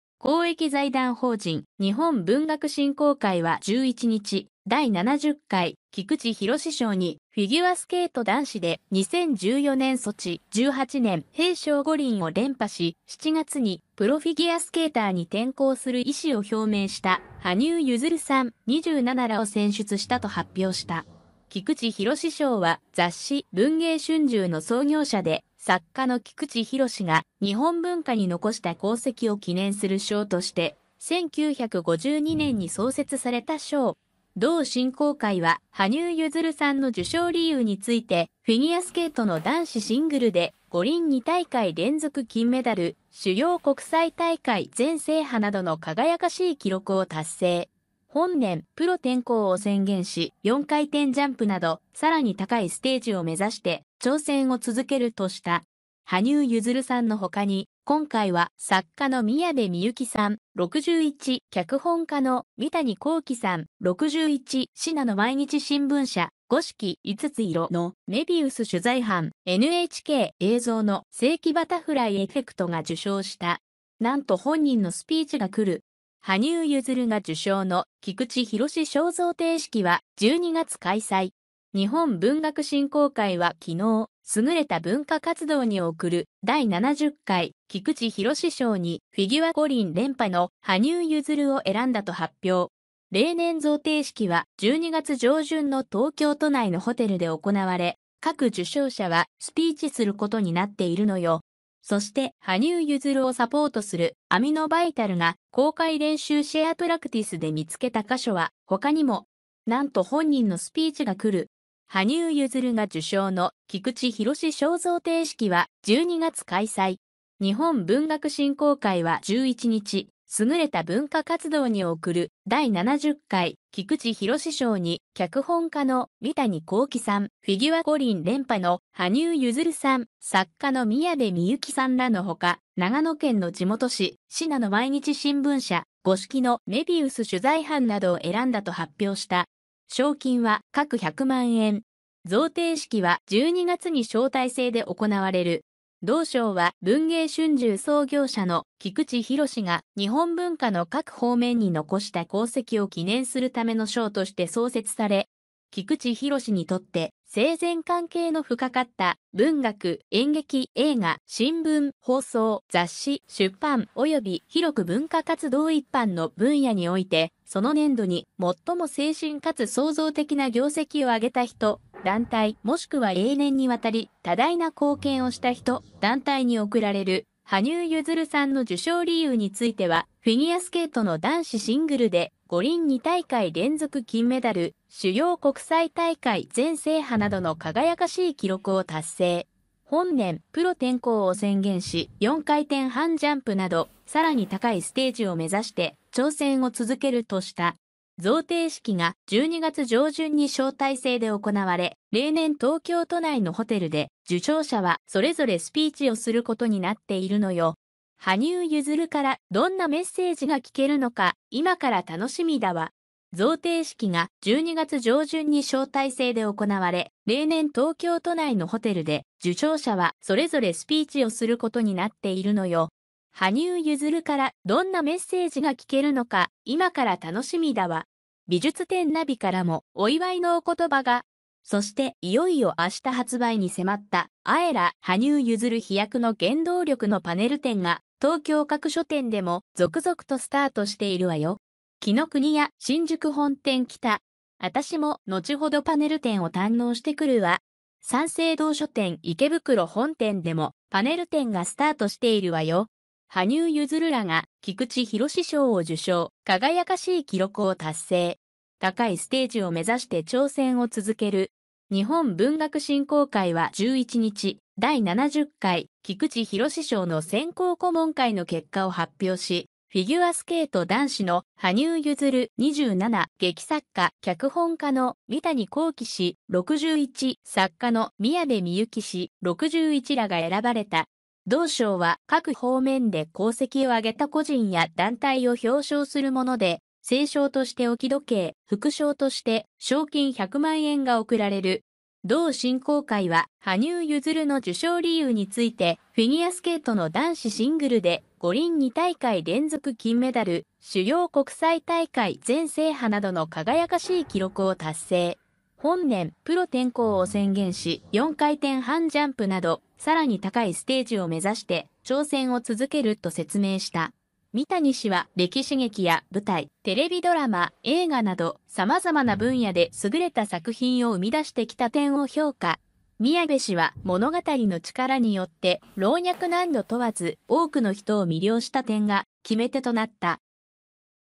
羽生結弦さんに菊池寛賞「五輪2大会連続金メダルなど輝かしい記録」宮部みゆきさん、三谷幸喜さんらも選出
公益財団法人・日本文学振興会は11日、第70回菊池寛賞に、フィギュアスケート男子で2014年ソチ、18年平昌五輪を連覇し、7月にプロフィギュアスケーターに転向する意思を表明した羽生結弦さん（27）らを選出したと発表した。

　菊池寛賞は雑誌「文藝春秋」の創業者で作家の菊池寛が日本文化に残した功績を記念する賞として1952年に創設された賞。同振興会は羽生さんの受賞理由について、「フィギュアスケートの男子シングルで五輪2大会連続金メダル、主要国際大会全制覇などの輝かしい記録を達成。本年プロ転向を宣言し、4回転ジャンプなど、さらに高いステージを目指して挑戦を続ける」とした。

　羽生さんのほかに、今回は作家の宮部みゆきさん（61）、脚本家の三谷幸喜さん（61）、信濃毎日新聞社「五色（いつついろ）のメビウス」取材班、NHK「映像の世紀バタフライエフェクト」が受賞した。